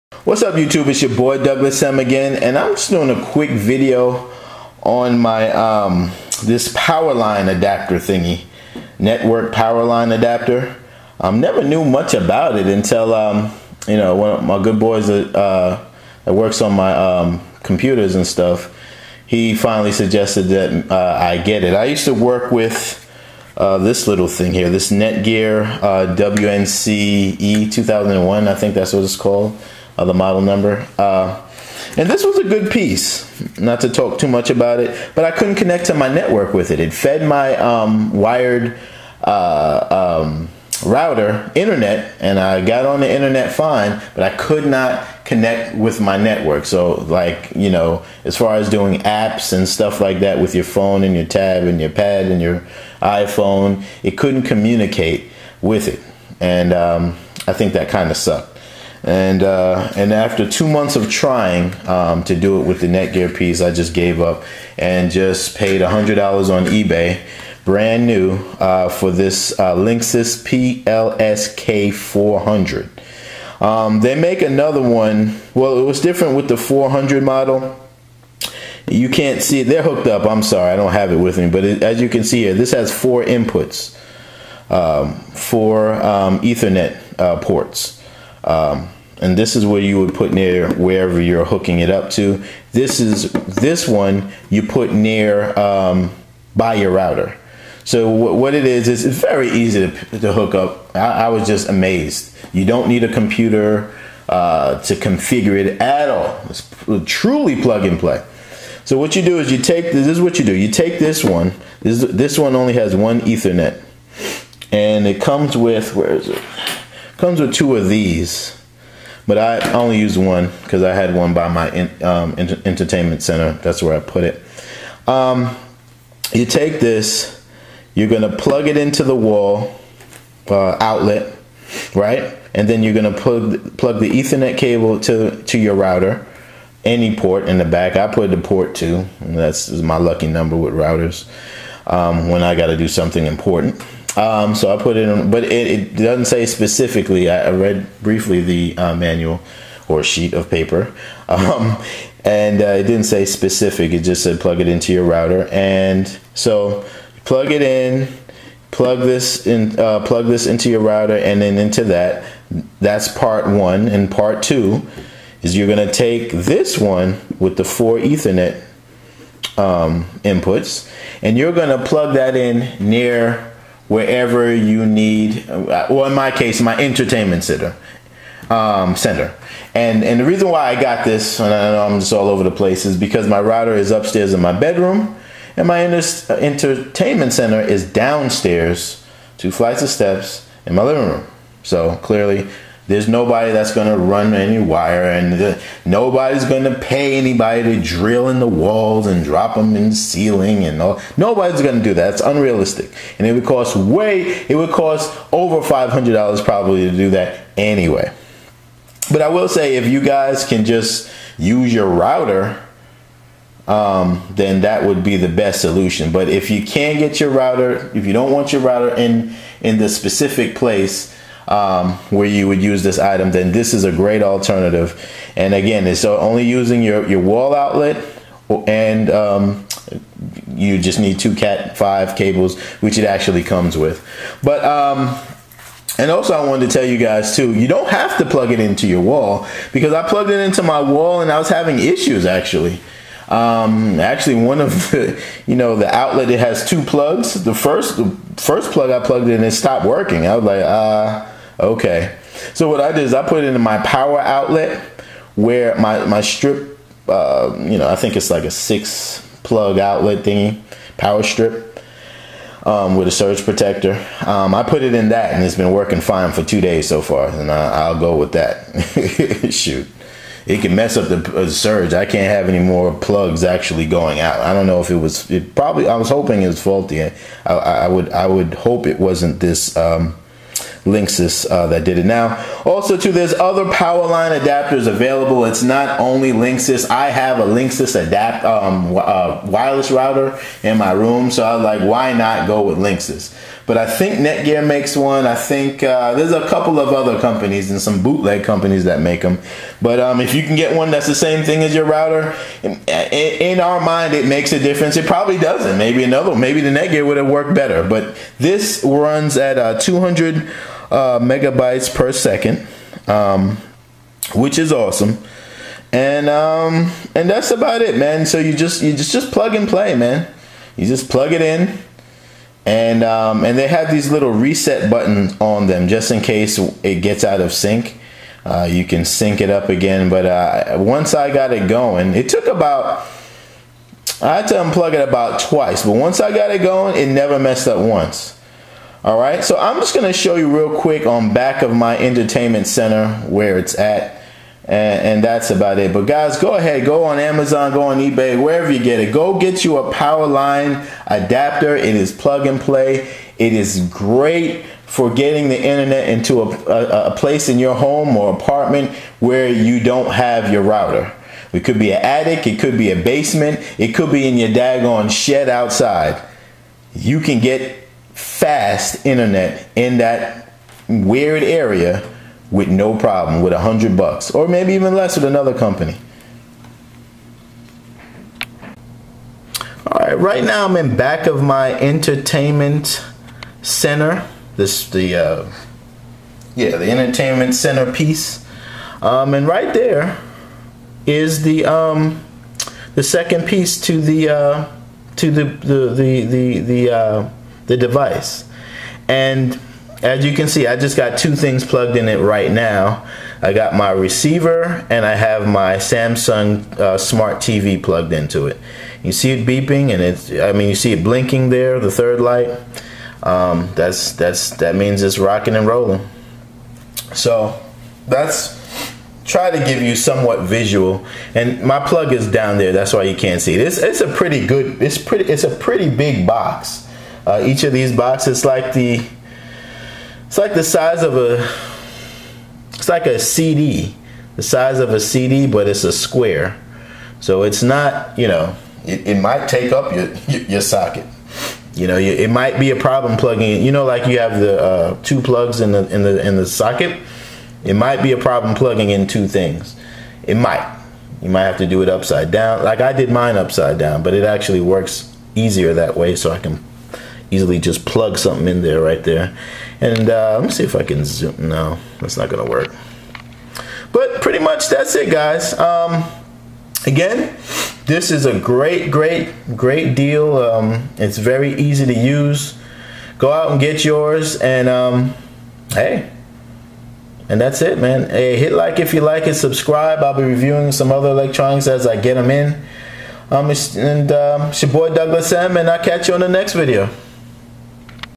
CISCO PLSK400 POWER LINE AV 4-PORT NETWORK ADAPTER KIT
CISCO PLSK400 POWER LINE AV 4-PORT NETWORK ADAPTER KIT. THIS IS IDEAL FOR DEVICES THAT DON'T HAVE WIFI,OR HAVE A WEAK WIFI SIGNAL